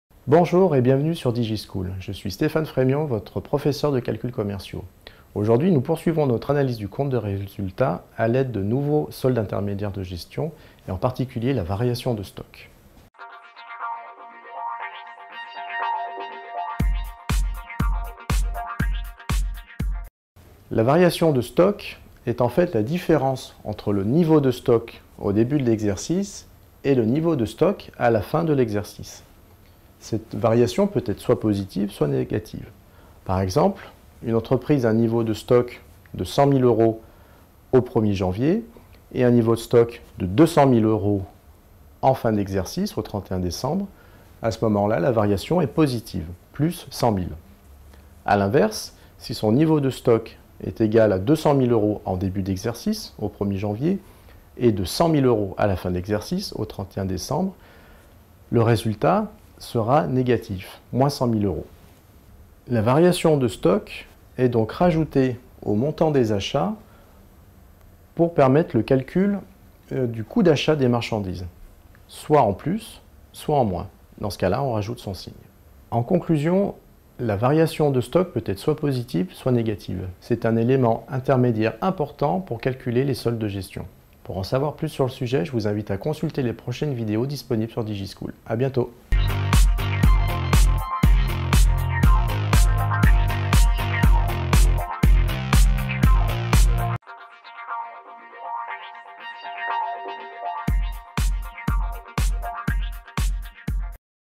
Analyser un compte de résultat, les SIG, la variation de stock - BTS Ci
➽ Retrouvez ce cours de calculs commerciaux niveau BTS CI sur l'analyse du compte de résultat en version pdf sur notre

Notre professeur de calculs commerciaux, Stéphane FREMION, dédie son cours d'analyse du compte de résultat aux soldes intermédiaires de gestion (SIG), et voit ici la notion de variation de stocks. Elle représente la différence entre le niveau de stock au début de l'exercice et le niveau de stock à la fin de l'exercice, et peut être positive ou négative.

pour retrouver l'ensemble de nos documents !